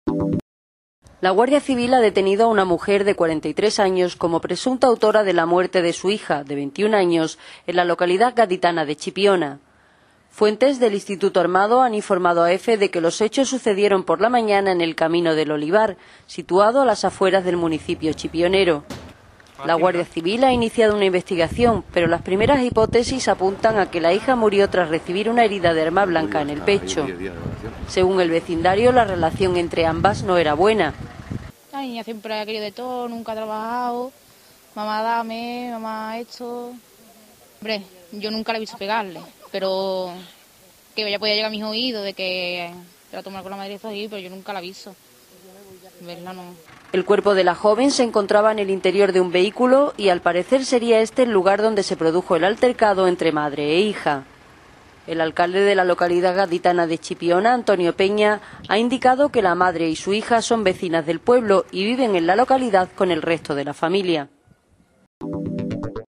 Detenida por matar a su hija en Chipiona
Cádiz, 13 ago (EFE)(Imagen: Román Ríos) - La Guardia Civil ha detenido a una mujer de 43 años como presunta autora de la muerte de su hija de 21 años en la localidad gaditana de Chipiona.

 Fuentes del Instituto Armado han informado a Efe que los hechos sucedieron en torno a las once de la mañana en el camino del Olivar situado a las afueras del municipio chipionero.